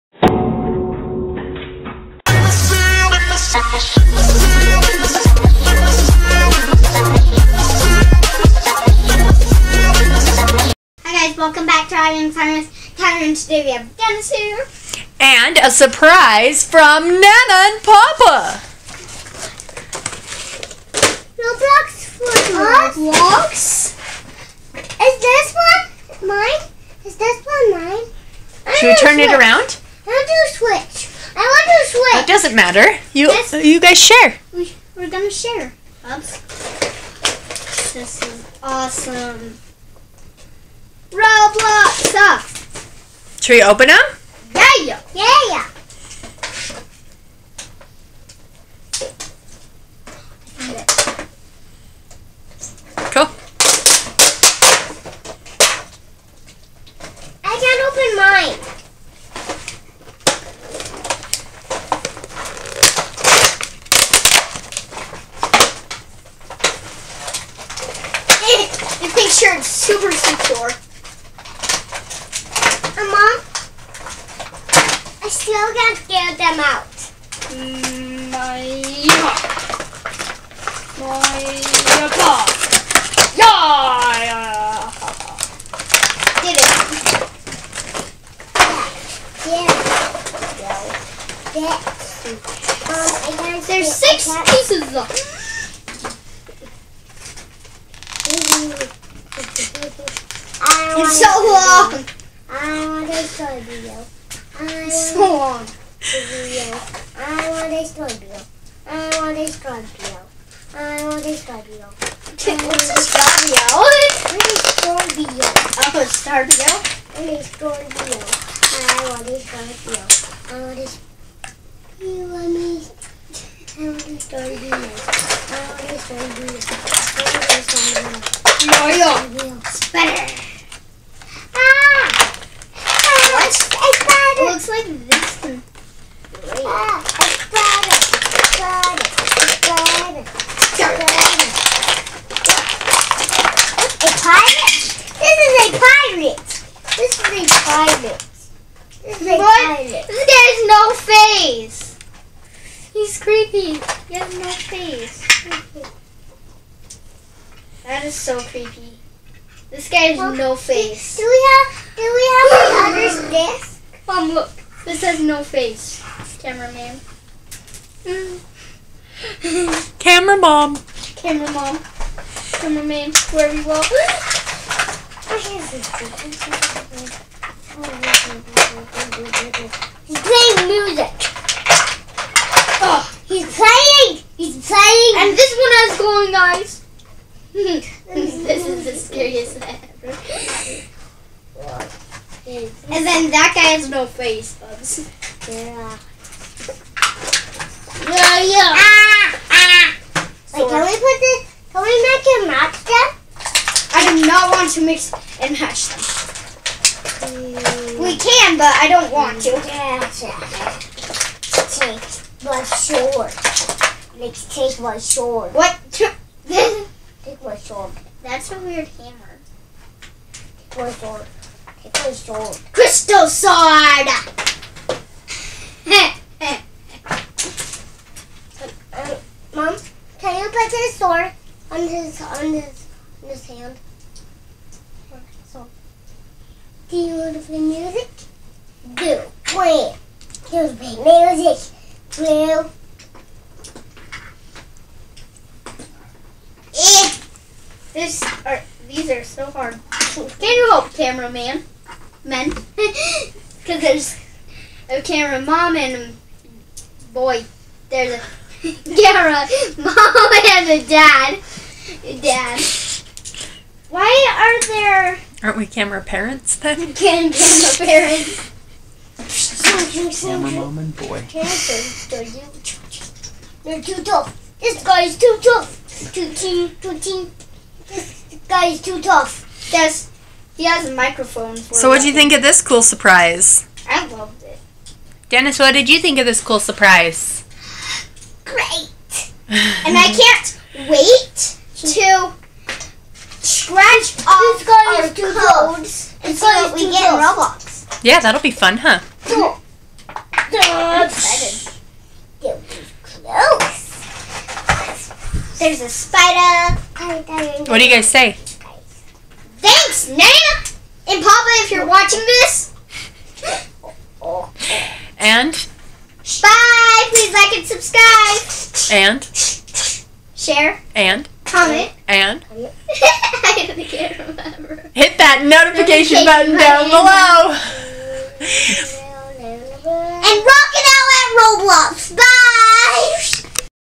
Opening some roblox toys with Tyler and Dennis
Nana and papa brought gifts for the boys. We thought it might be fun to share them with you guys too!
Mom says sorry to all our subscribers for not getting to the editing till now. Shes been sick for a couple weeks. But we are on the mend :)